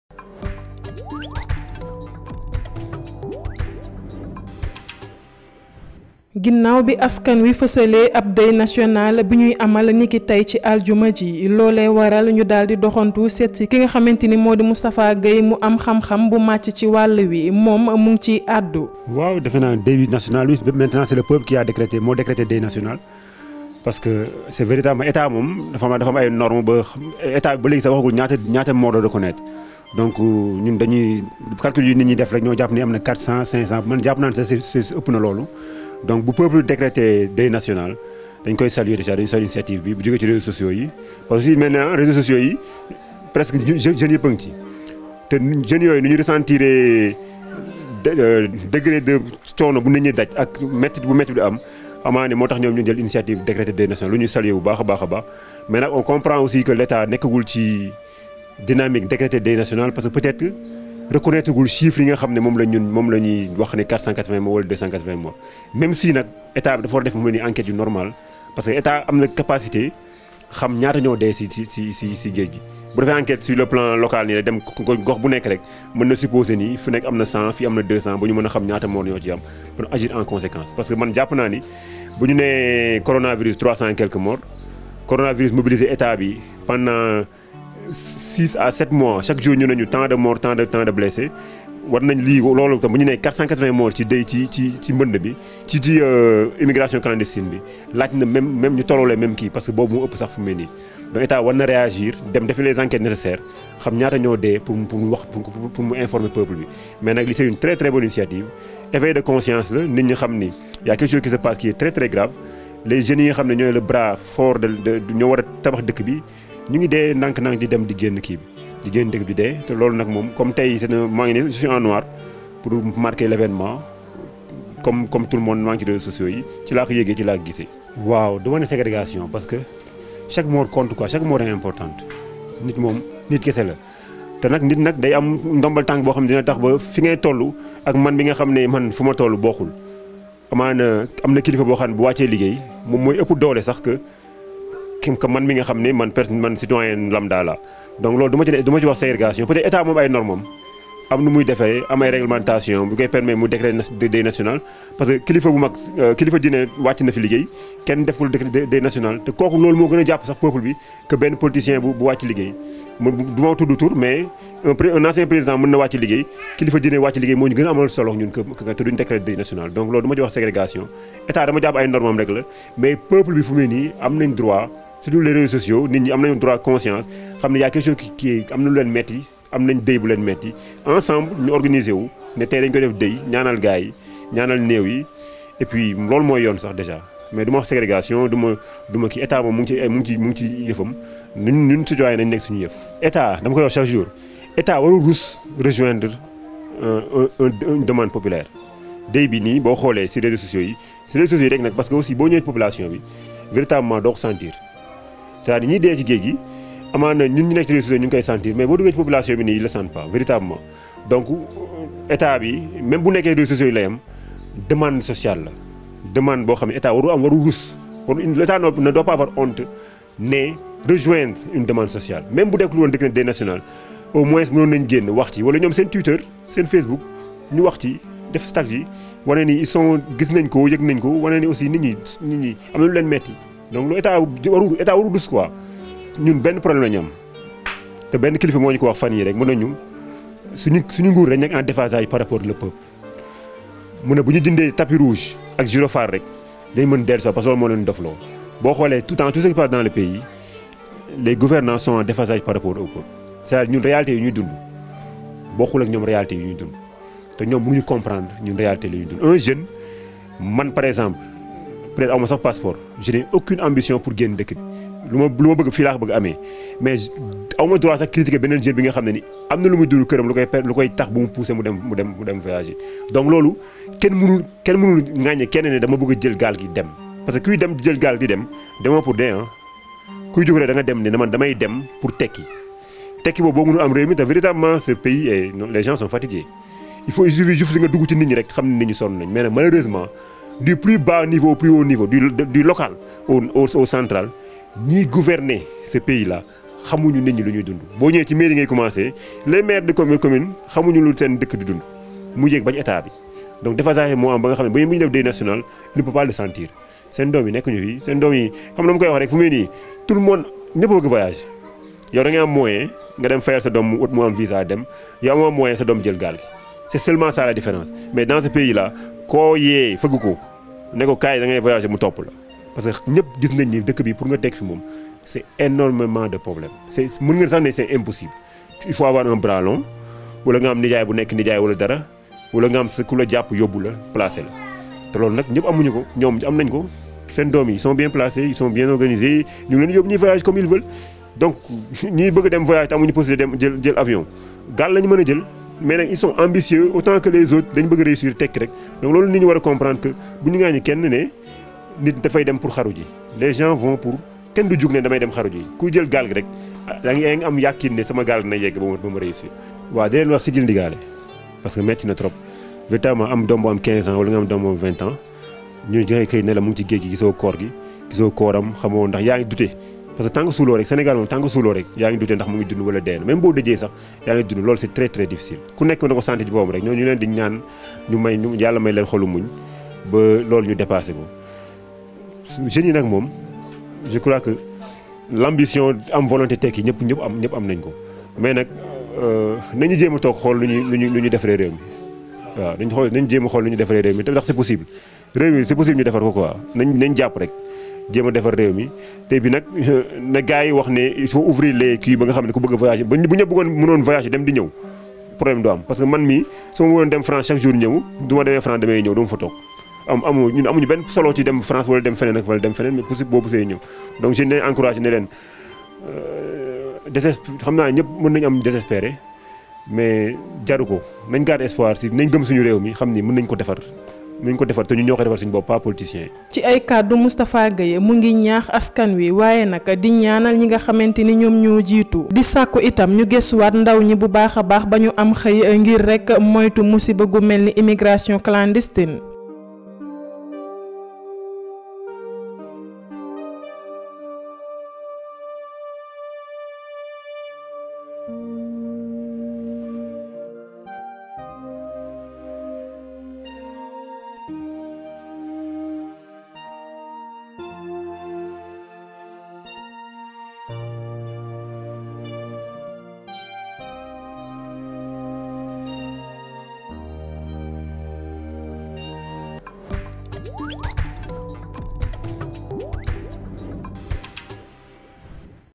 Deuil national: Moustapha Gueye valide et ajoute: "l’Etat doit aussi revoir sa posture…"
Suite à la vague de réactions découlant des morts annoncées dans les tentatives de migrations clandestins, Leral Tv s’est rapprochée des populations pour leur avis sur ce triste phénomène. Interrogé pour son appréciation sur cette initiative,  Moustapha Gueye valide cet élan, mais sur la posture de nos autorités…